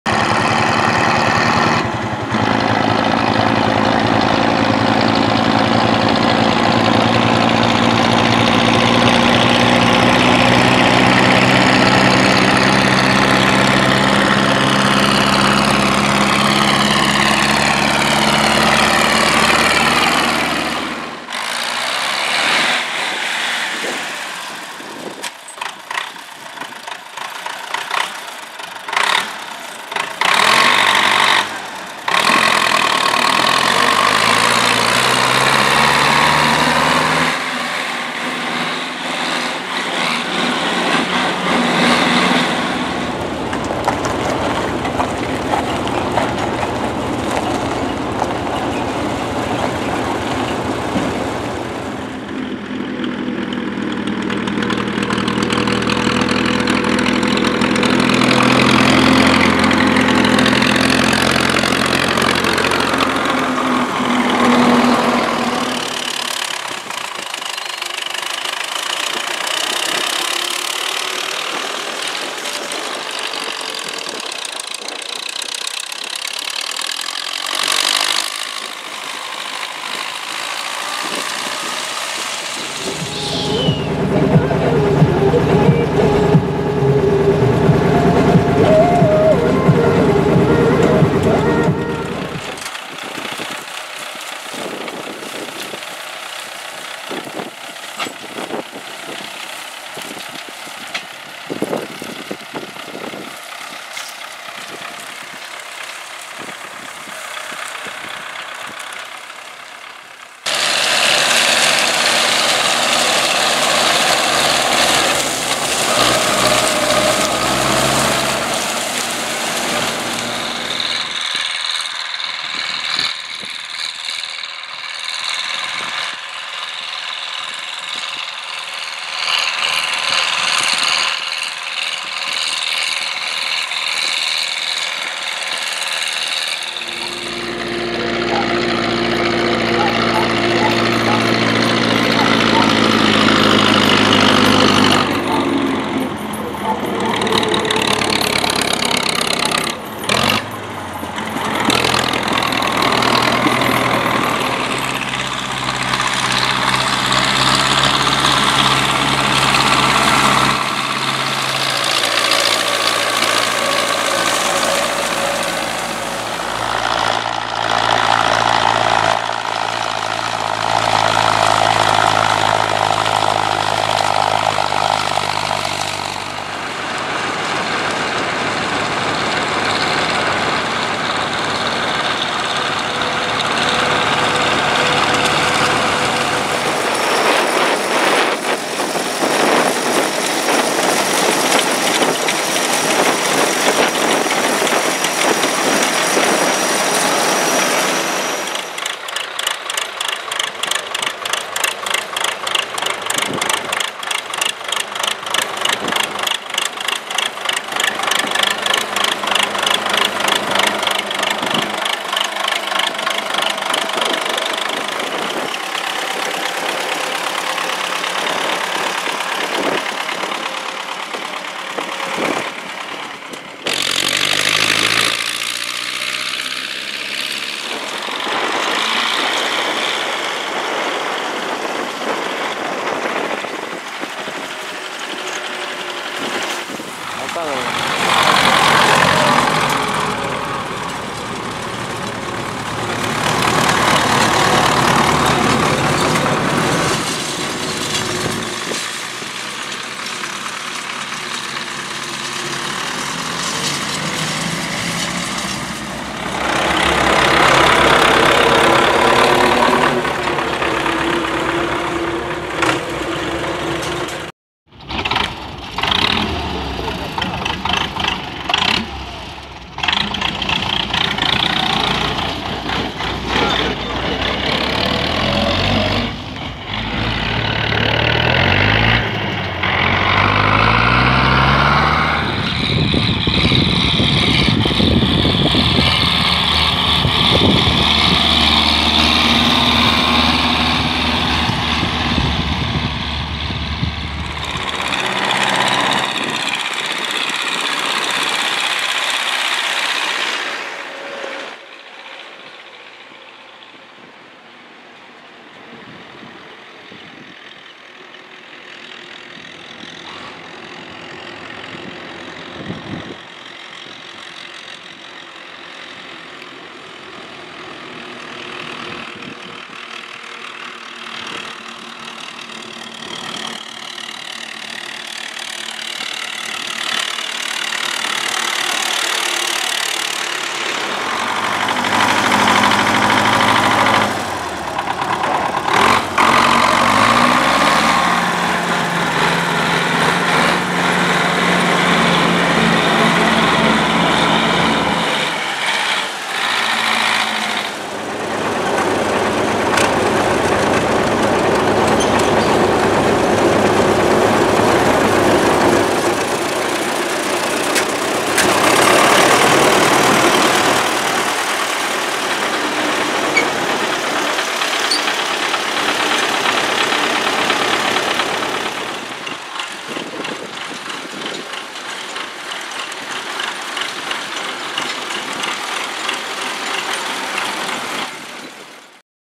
Мтз 82
Как работают тракторы мтз 82 в нашем хозяистве

Heciul Nou , Sîngerei
ALODIU-NORD S.R.L.